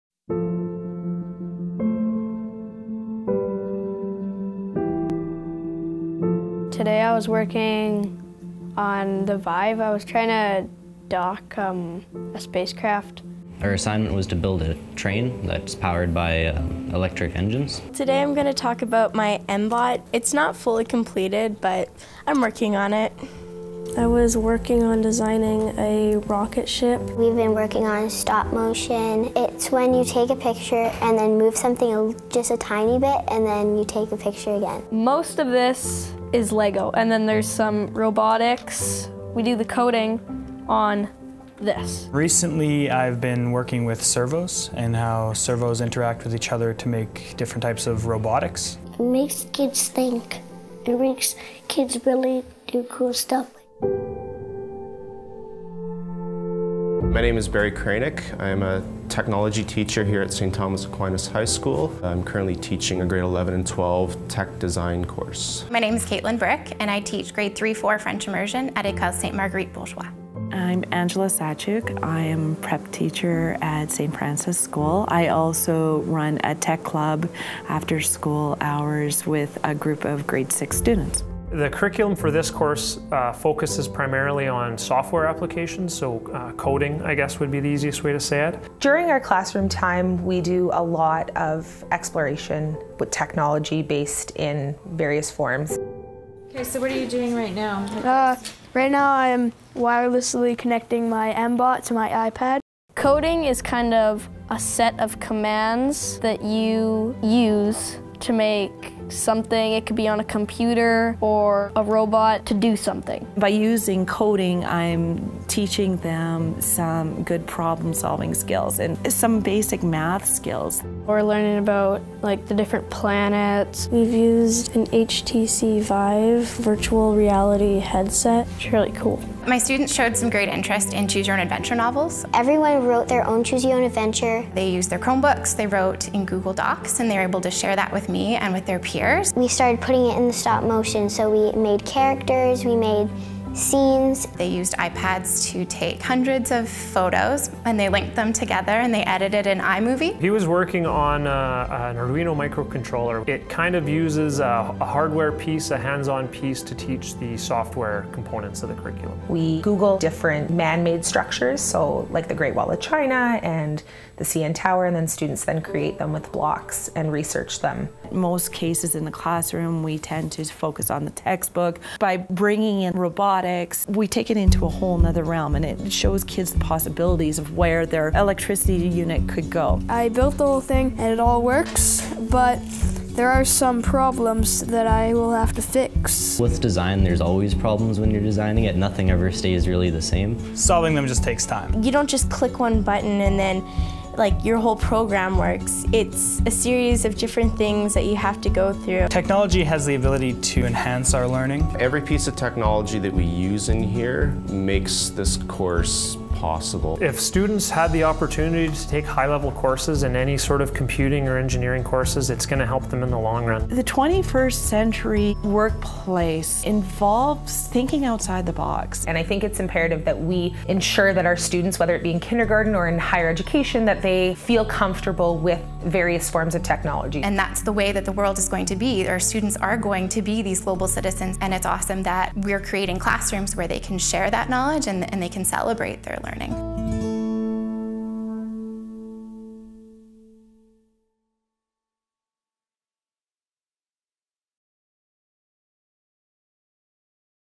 KCDSB and TNCDSB - High-Tech Students for a High-Tech Tomorrow
"The 21st century workplace involves thinking outside the box."
We are very excited to have partnered with The Northwest Catholic District School Board to give you a look inside our high-tech learning studios where students are preparing today for the world of tomorrow.